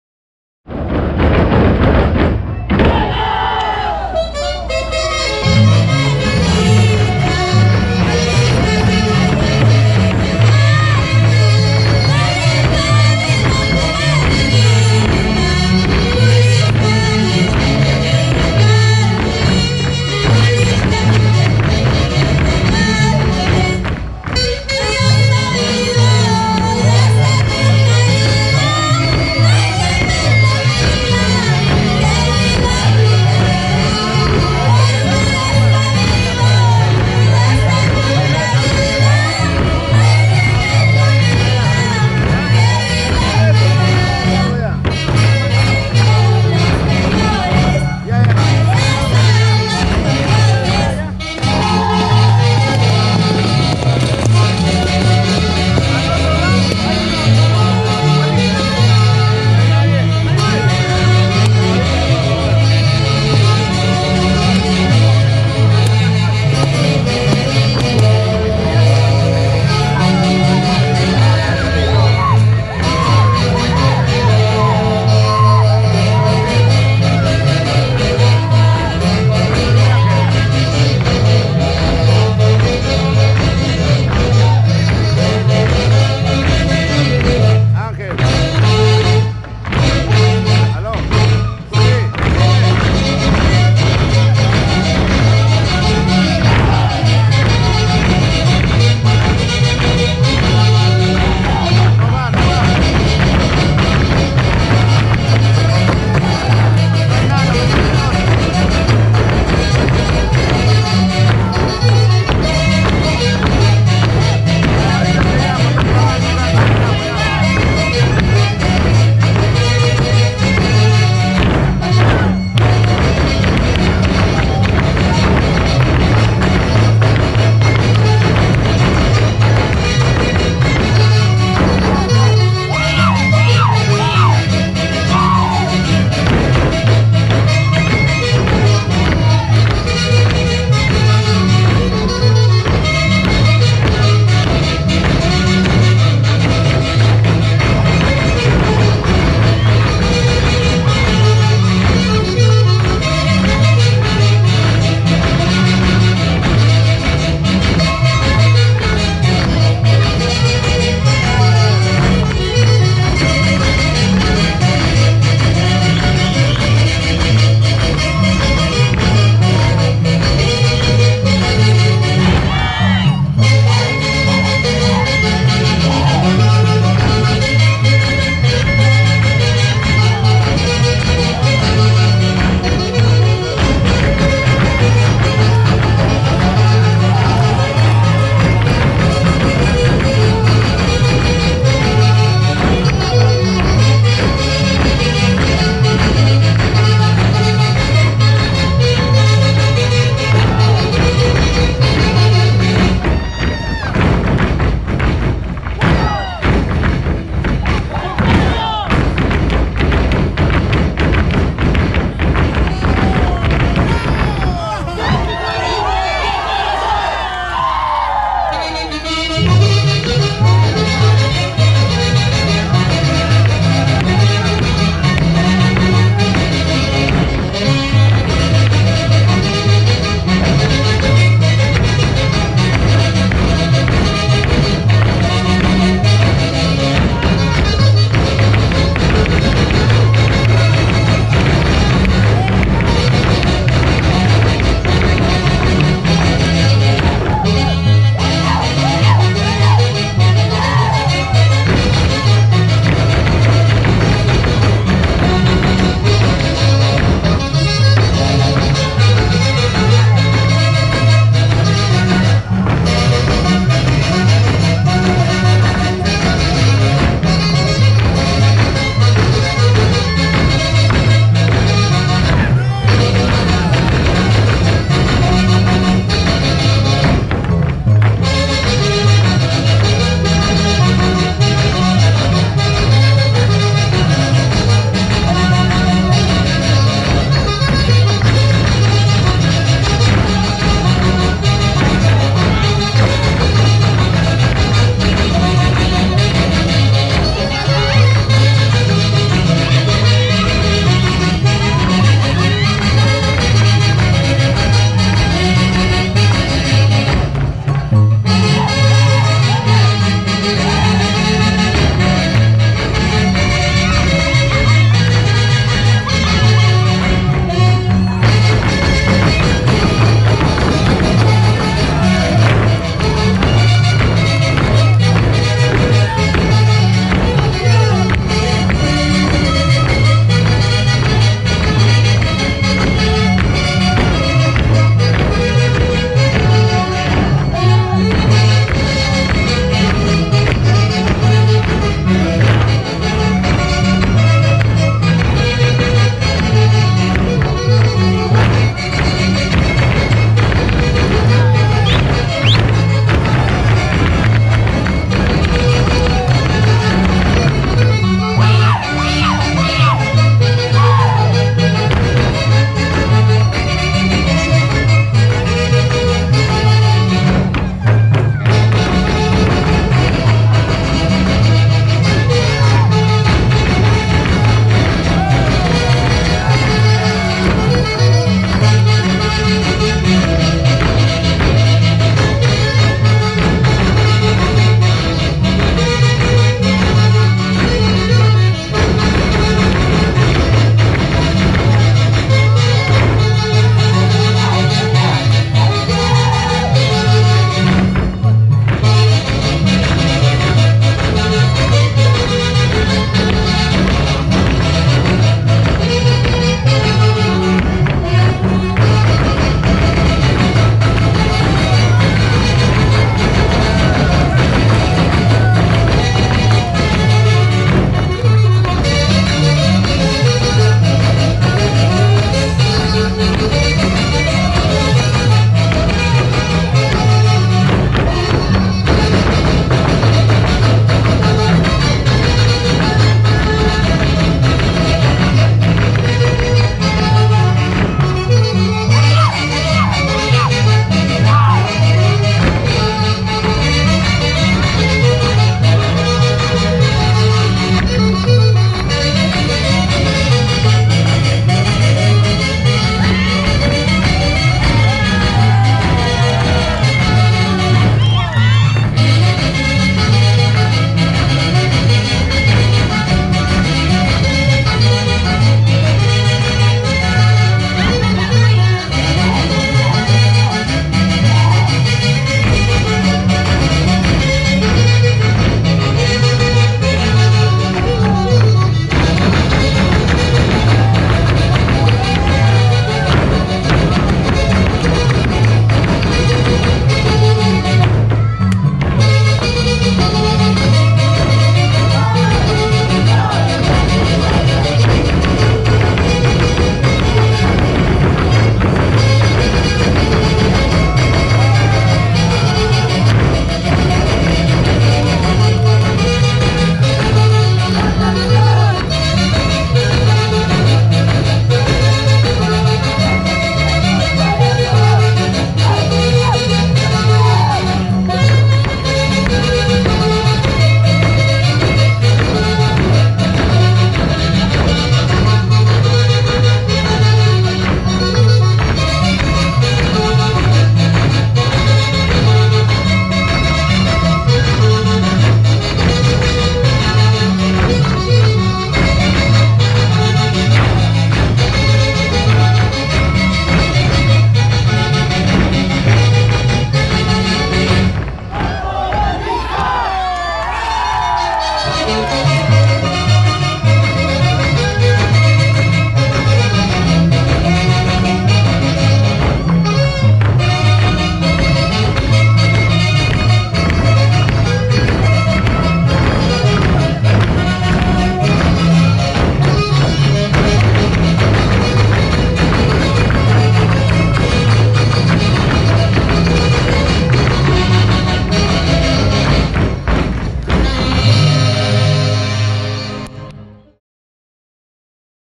ALIANZA PUCARÁ- HUAYLARSH MODERNO TUKILLA PERÚ
TUKILLA PERÚ  ..  PRESENTA AL CONJUNTO DE  HUAYLARSH MODERNO ALIANZA PUCARÁ EN FRAMA 2019